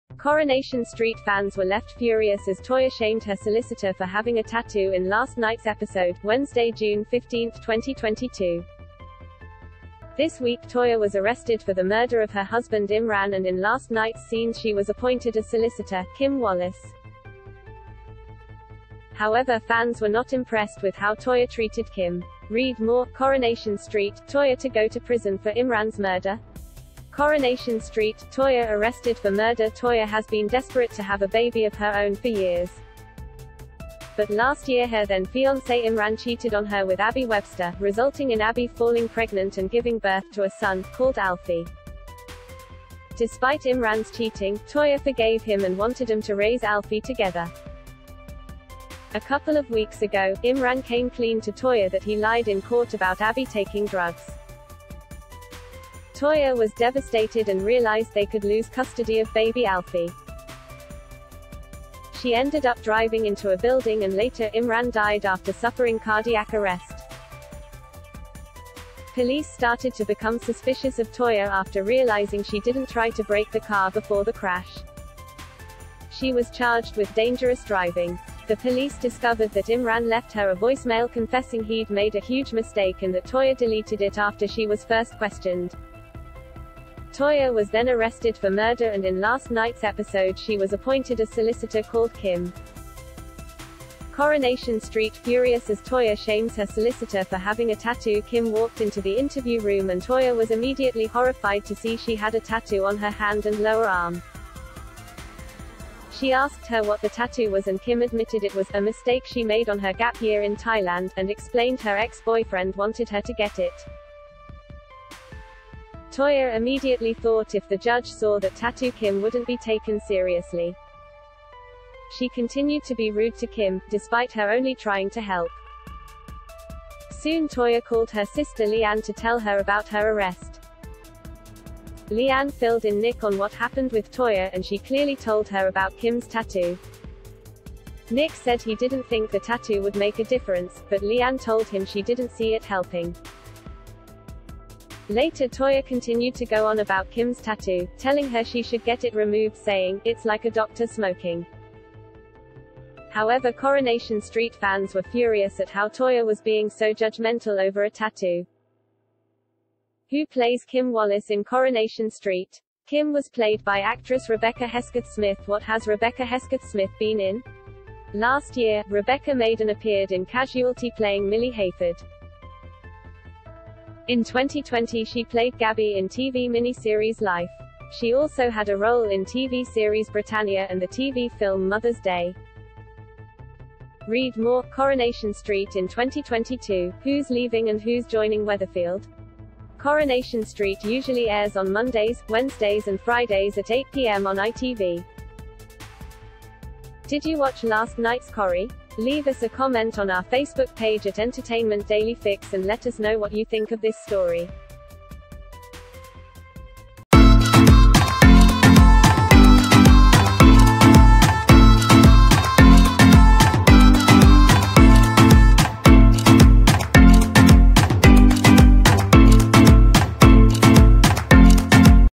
Furious Coronation Street fans slam Toyah as she ‘shames’ her solicitor over a tattoo
Coronation Street fans were left furious as Toyah shamed her solicitor for having a tattoo in last night’s episode (Wednesday, June 15 2022).  This week Toyah was arrested for the murder of her husband Imran and in last night’s scenes she was appointed a solicitor, Kim Wallis.  However fans were not impressed with how Toyah treated Kim.   Coronation Street: Toyah arrested for murder  Toyah has been desperate to have a baby